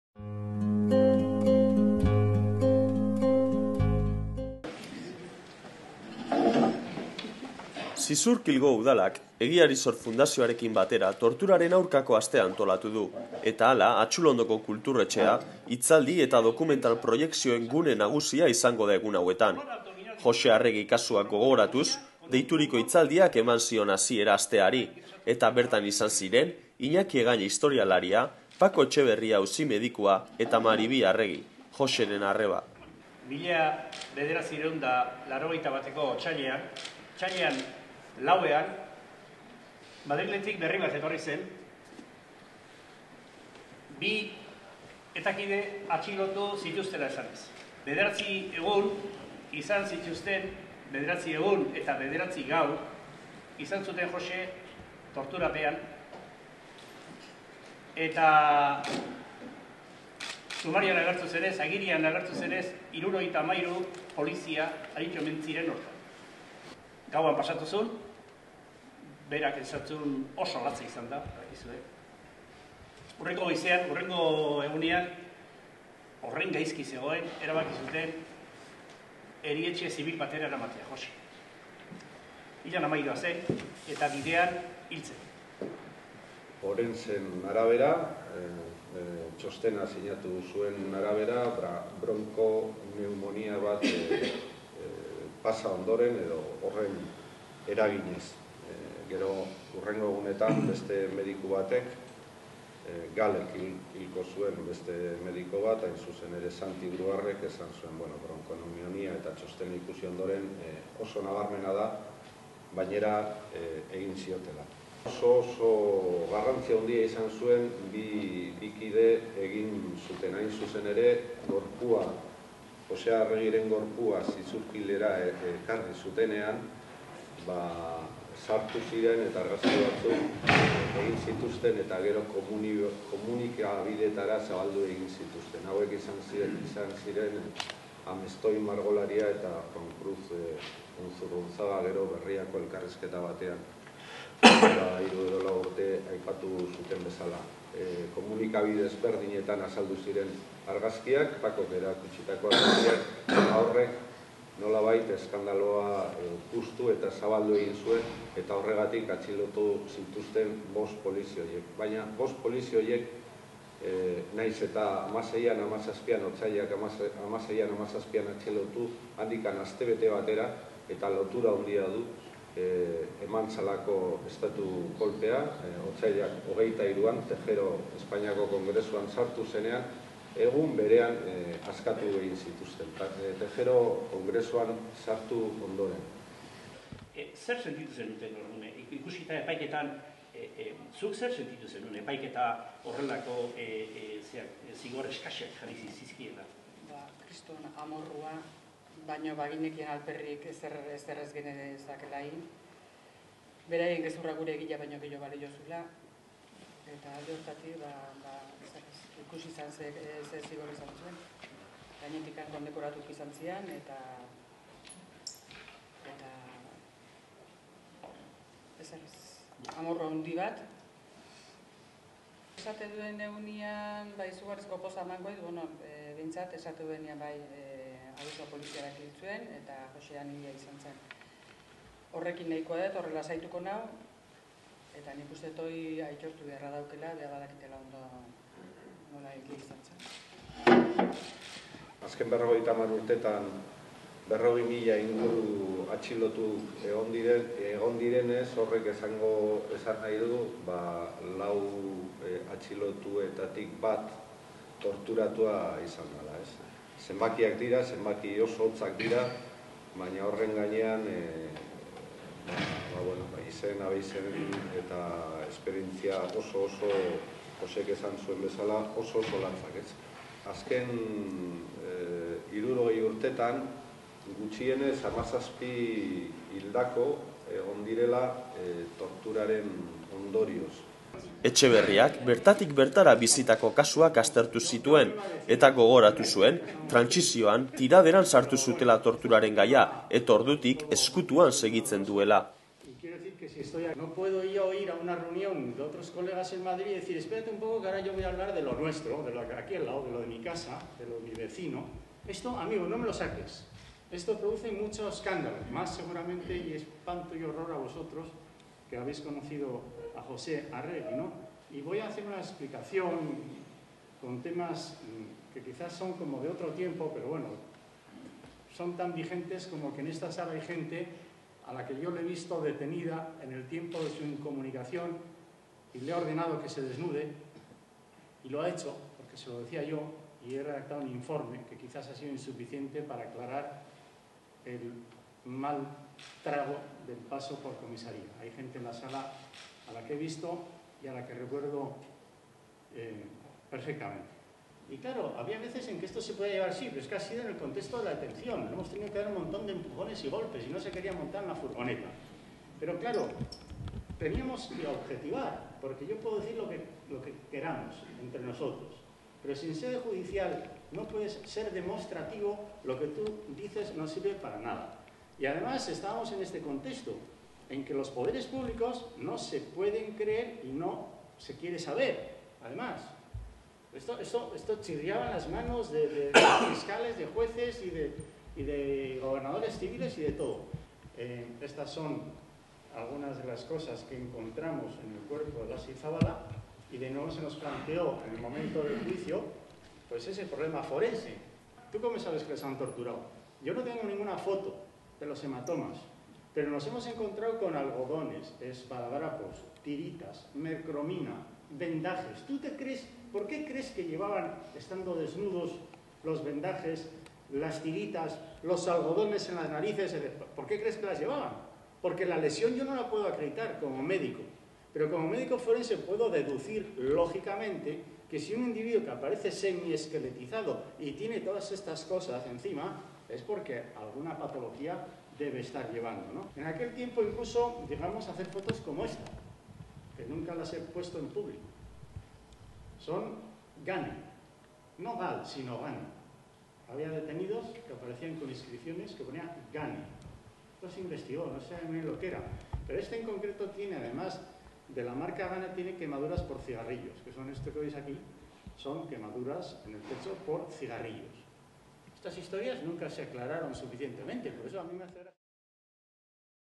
BERRIA.
Torturaren aurkako astea antolatu dute Zizurkilgo Udalak eta Egiari Zor fundazioak. "Joxe Arregi kasua gogoratuz" hitzaldiarekin hasi zuten aurreneko jardunaldia. Bertan izan ziren Iñaki Egaña, Francisco Etxeberria eta Maribi Arregi.